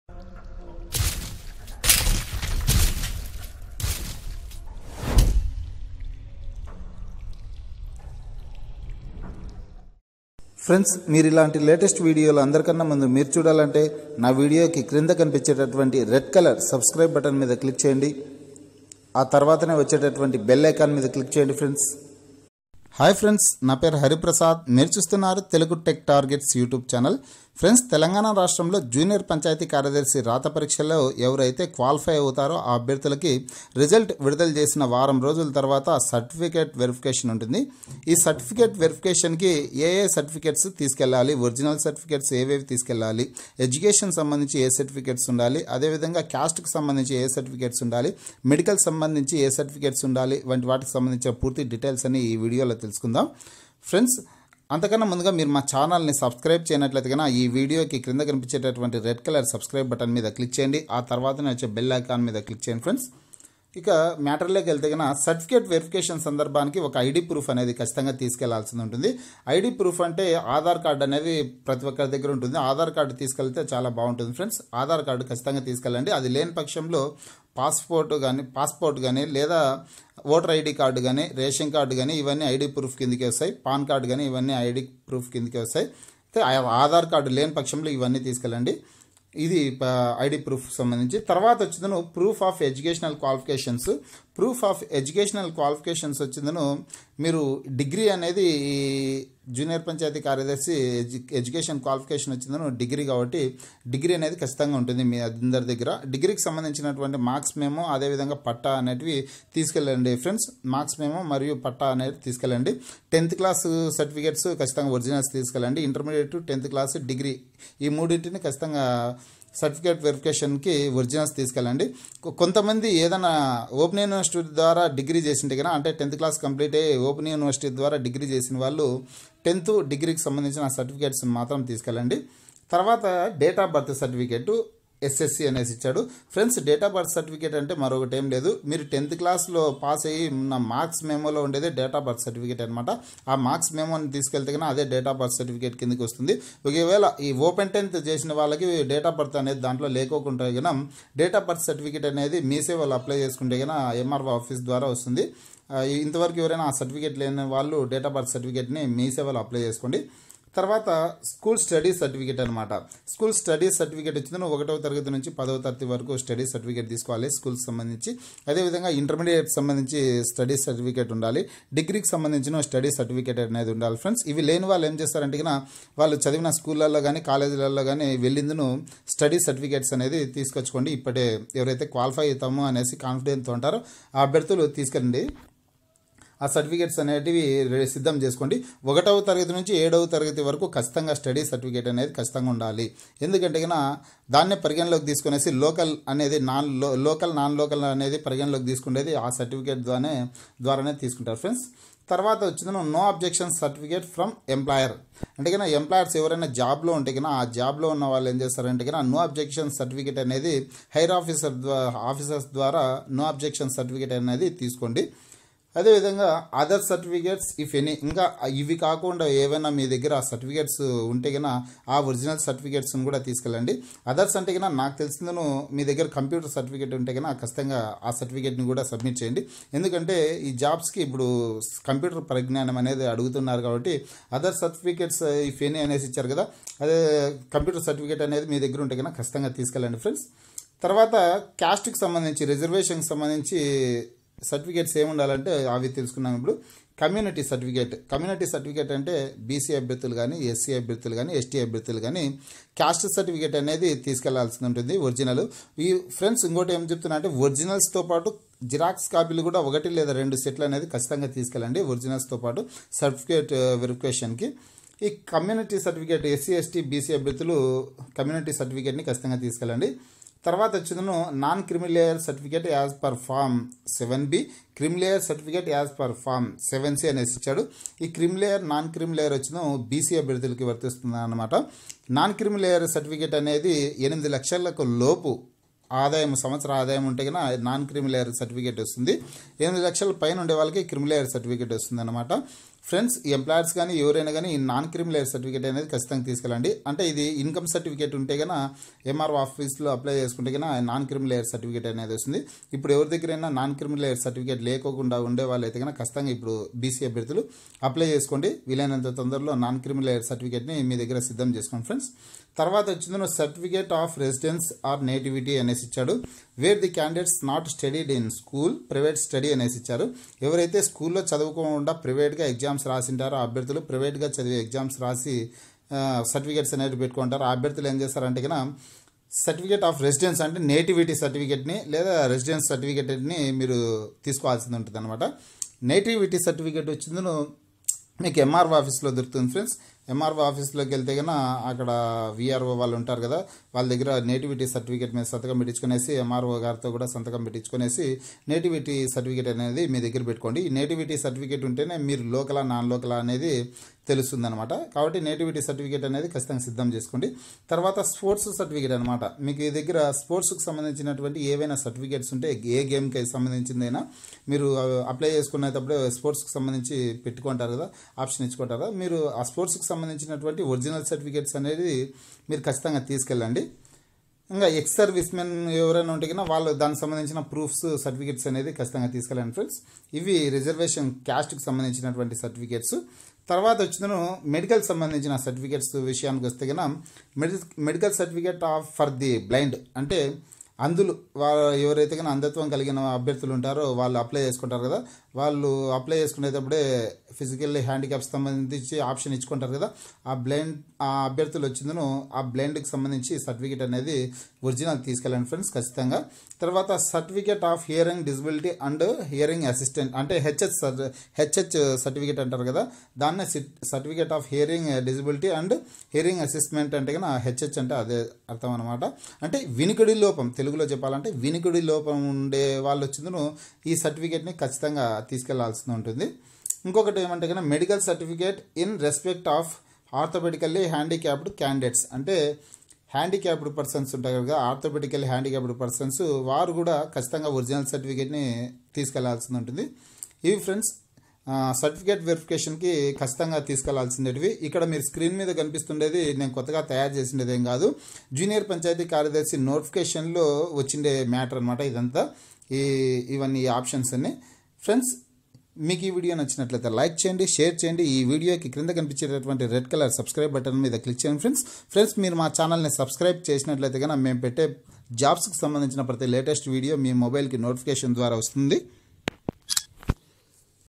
Required certificates for TS Junior Panchayat Secretary Jobs 2018 Certificate verification
Watch this video: Required certificates for TS junior panchayat secretary Recruitment 2018 certificate verification
   The following certificates must be kept ready by the candidates for the
purpose of verification after final selection
1). Aadhar card or other ID proof as indicated in application.
2). Proof of Educational Qualifications.
3). Date of Birth Certificate / S.S.C.
4). School Study Certificate.
5). No Objection Certificate from Employer (if anywhere employed)
 6). Other certificates if any.
   The following Certificates should be obtained from Govt. of Telangana State in
prescribed proforma for the purpose of verification.
1). Community Certificate.
2). Non-Creamy Layer Certificate as per Form- VIIB / Creamy Layer Certificate
as per Form-VIIC.
3). Certificate of Residence / Nativity (where the Candidates not studied in
School / Private Study).
4). Sports Certificate.
5). Ex-servicemen.
    The following Certificates (whichever is applicable) should be obtained
from Competent Medical authority for the purpose of verification.
1) Medical Certificate for the Blind.
2) Certificate of Hearing Disability and Hearing Assessment.
3)Medical Certificate in respect of Orthopedically Handicapped Candidates.